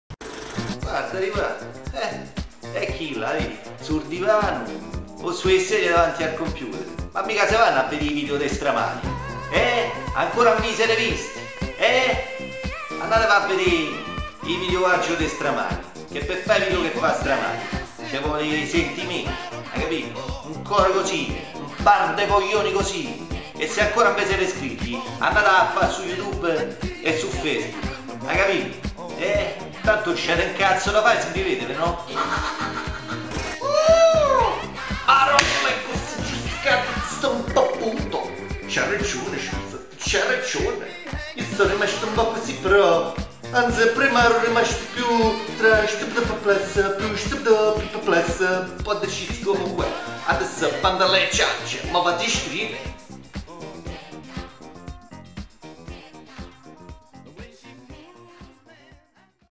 Spot STRA MALI (con omaggio a Tomas Milian e Diego Abatantuono)
Simpatico invito ad iscrivervi al mio canale youtube "STRA MALI" dove si omaggiano con dei video gli attori e colonne sonore dei polizieschi anni 70' e delle commedie italiane anni 80/90...vi aspetto anche sulla pagina Facebook!!! ;)